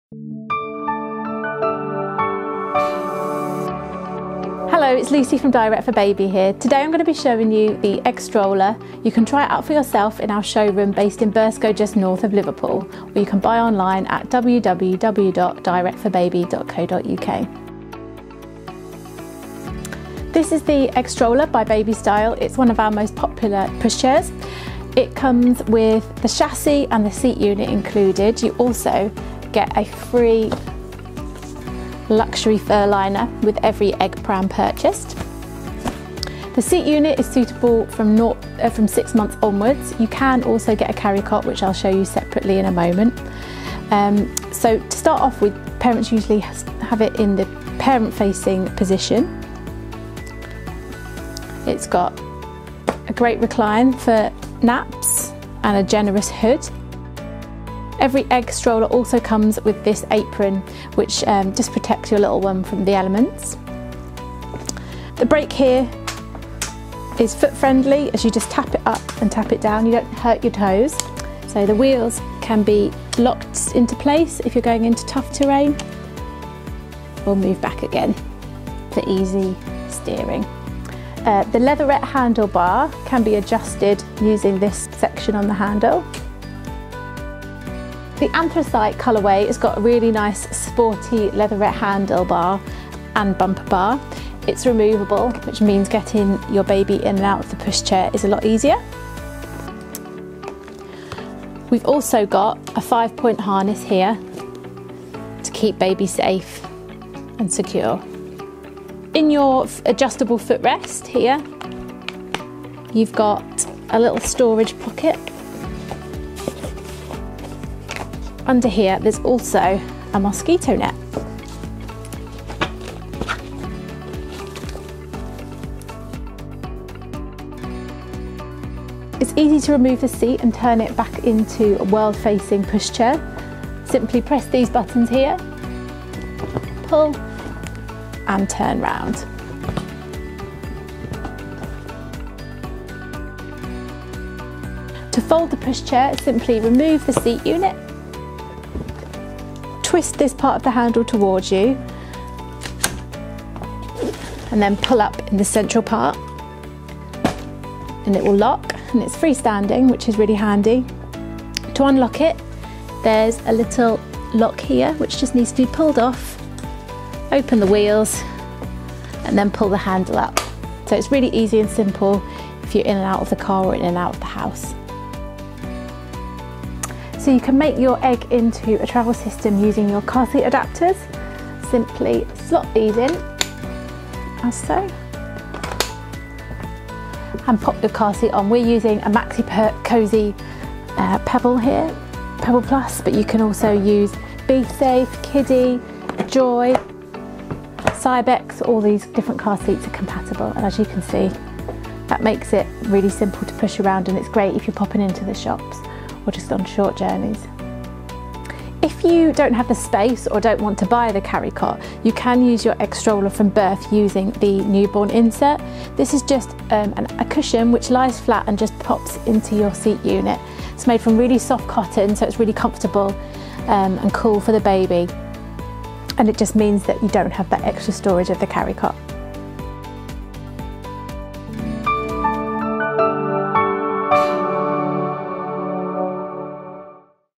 Egg Stroller with Newborn Insert in Anthracite - demo from Direct4baby
Shaped like its name suggests, the egg® stroller’s curved lines will provide a safe and stylish buggy for you and your baby. The egg® stroller has been designed in the UK with the style conscious parent in mind. Soft touch fabrics create smooth curves and continuous lines to give the egg® a luxurious, stylish look. Outstanding engineering creates the perfect ride for baby and ultimate ease of use for parents. The egg® has been fitted with Tru-Ride Technology® tyres which have been engineered for comfort, superior handling and added durability. The one-hand fold chassis is compact and freestanding when folded and the three position recline, parent and forward facing seat unit is spacious and comfortable.

The brand new 2019 Egg Special Edition stroller has been finished in soft fabrics and smooth curves for a luxurious look. The sleek designed baby transport system features a shiny chrome frame  with cool blue fabrics, plus leather handlebar and bumper bar, so both you and your baby can travel in style.

Instead of using the carrycot, you can use the egg® Newborn Insert instead. This is a soft padded insert which creates a lie-flat position in the seat unit for your newborn baby. Made from 100% soft cotton for ultimate comfort.